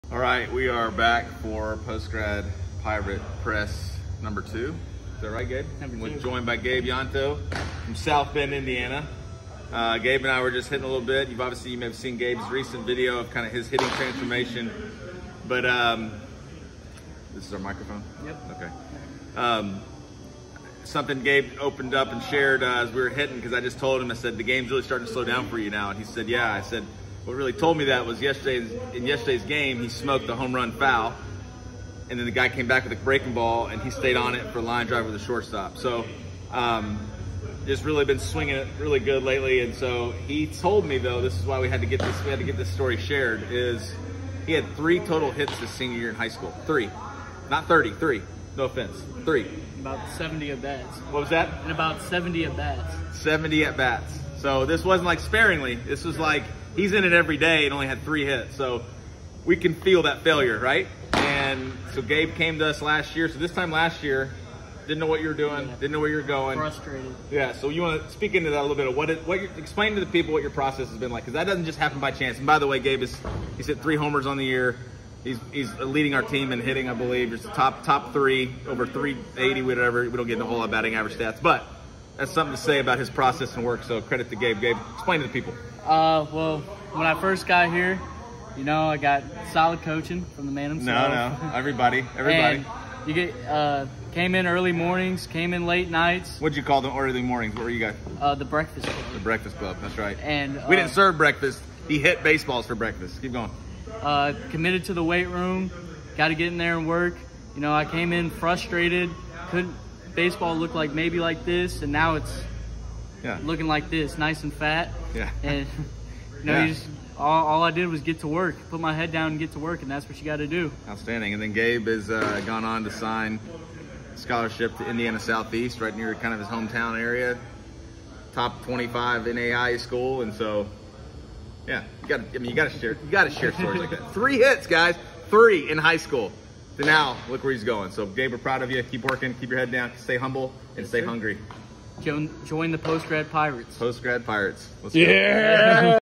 📰POST GRAD PRESS📰
Indiana University SE Commit,
Gabe Yonto from SouthBend, IN sharing some of his story and journey from where he was this time last year! 🤙

🎥 Cred: P.Taylor